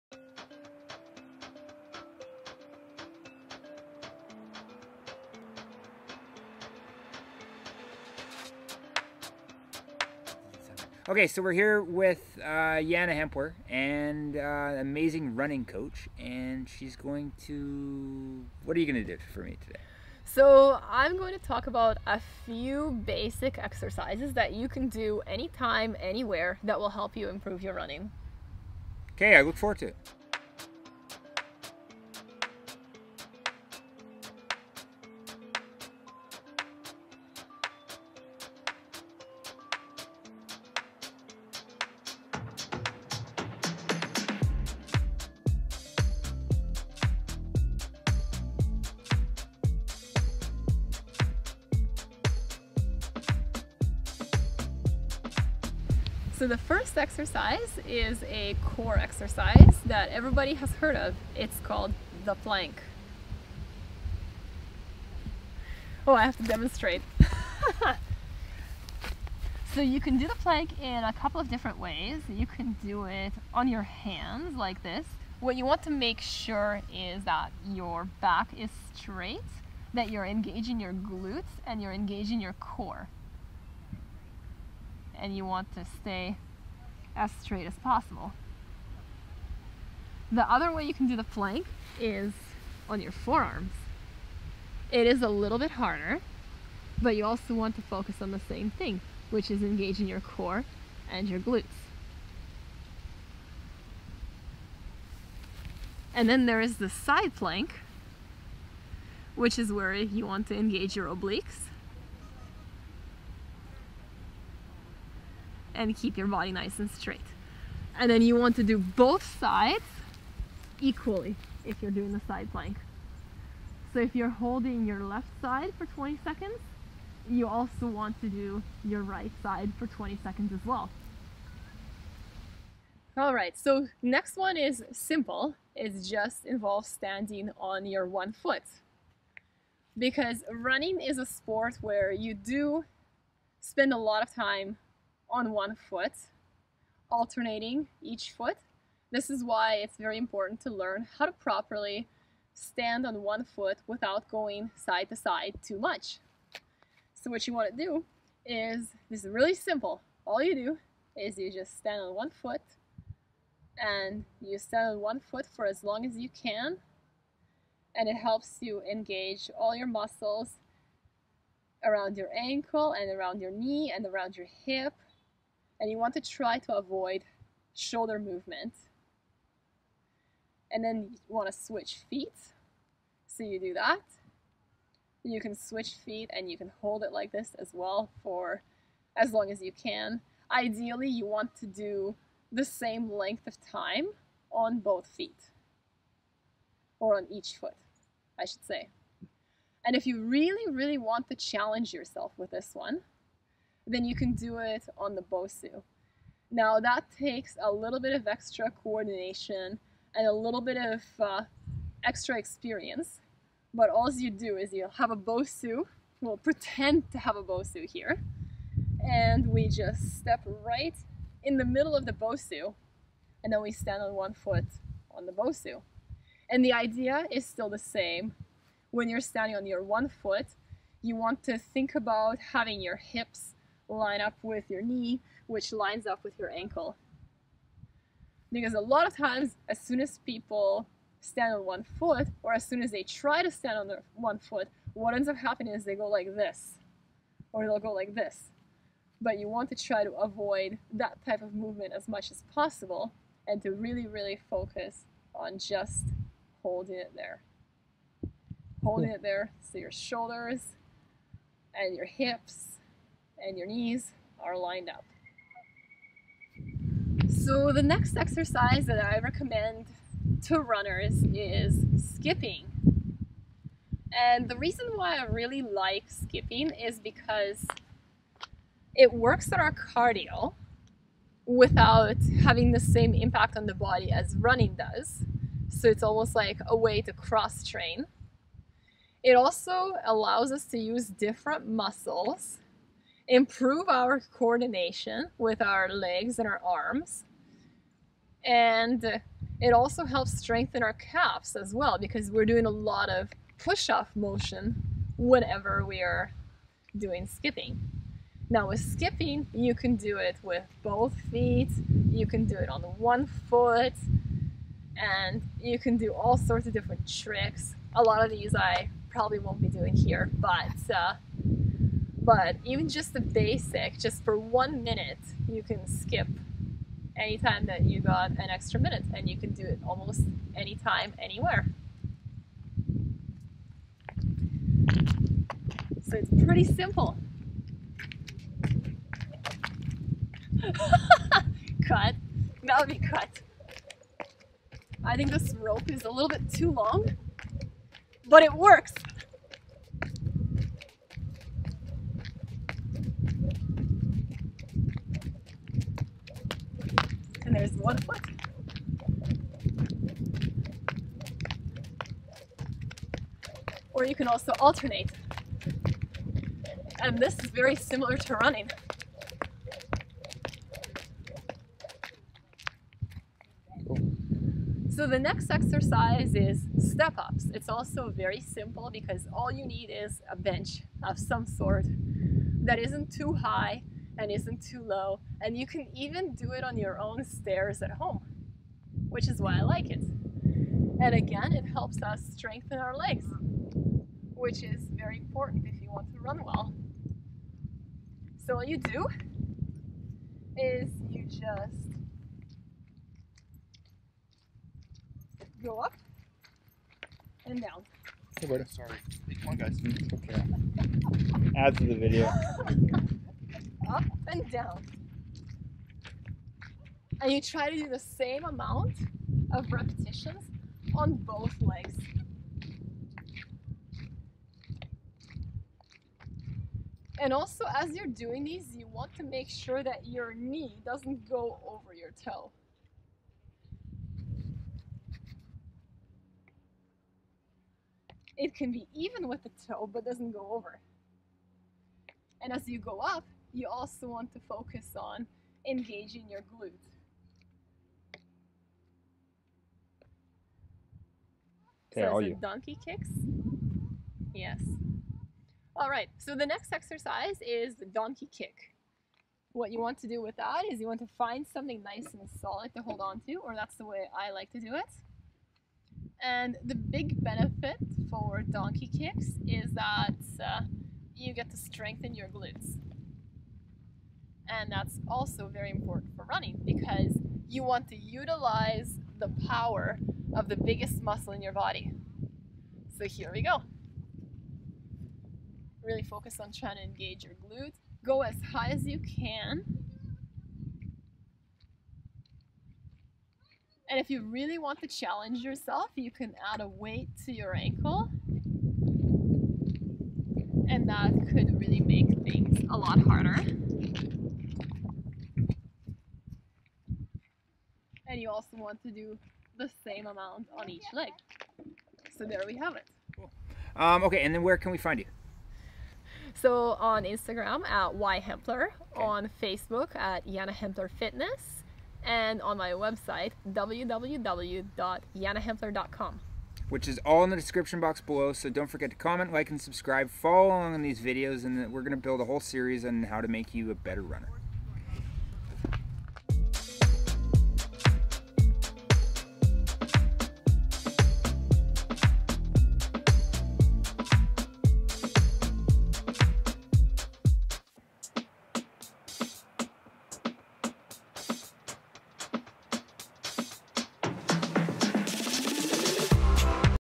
5 simple exercises to help you run faster | Day 76
These 5 simple exercises will help you to run faster, build a stronger core, build muscles away from running and keep you active and healthy.

If you would like to see a lot more from Yana Hempler.

★ CAMERA GEAR -

★ BIKING GEAR

To Buy Drip Fit Sweat Intensifer
To Buy Drip Fit Sweat Band